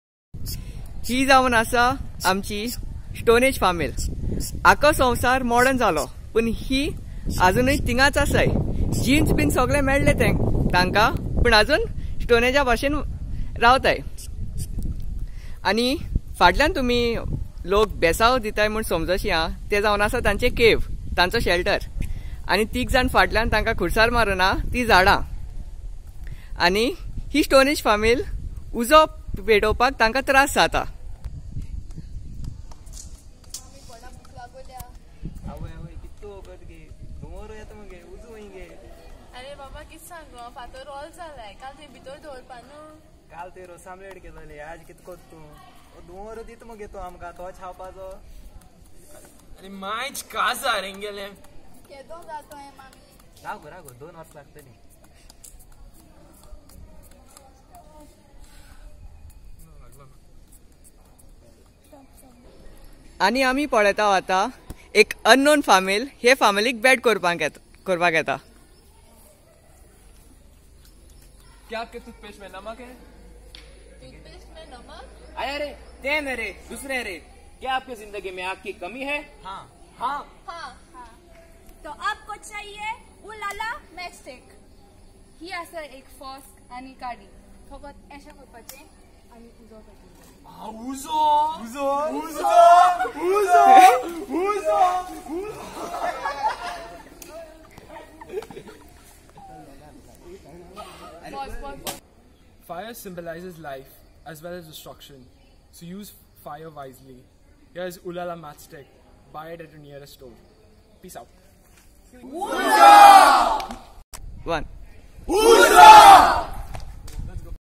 SFX Chicalim Parish Youth attend DYC Youth Connect 2019
At DYC Youth Connect 2019, participants were asked to produce an advertisement. This video is the outcome.